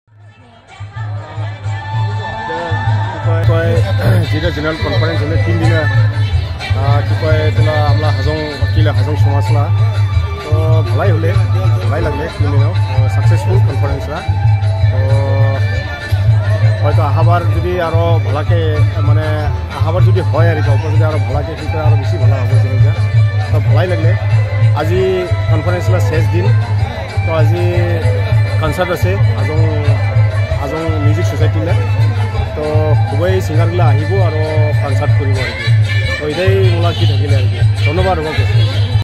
DASA HAJONG SHARE HIS VIEW II ALL INDIA & MEGHALAYA JOINED HAJONG CONFERENCE 2023 II TANGABARI
13TH JOINED CONFERENCE OF ALL INDIA & MEGHALAYA HAJONG WELFARE ASSOCIATION TANGABARI 2023
DASA HAJONG
DASA HAJONG SONGS